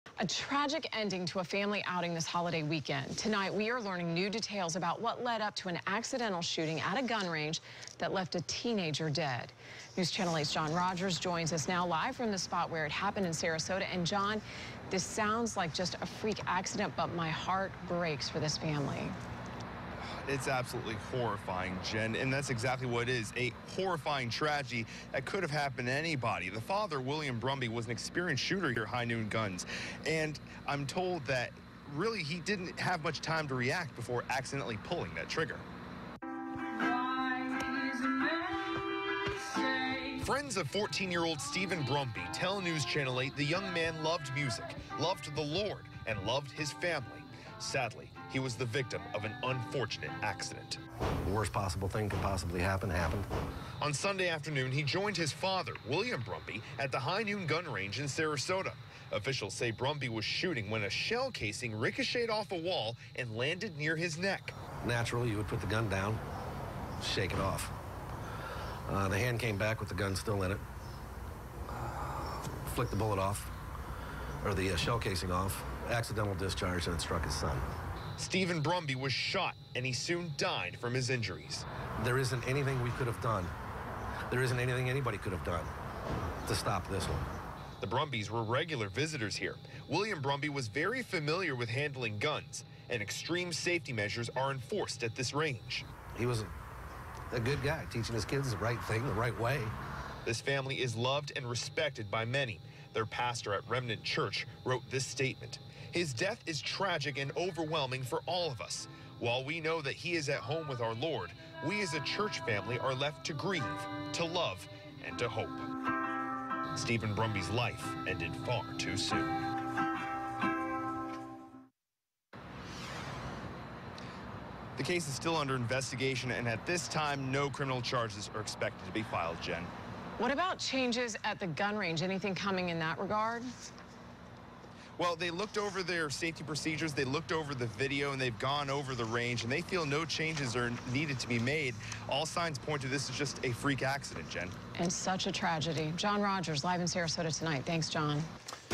Family outing turns tragic during holiday weekend
A father accidentally shot and killed his son at a Sarasota gun range Sunday.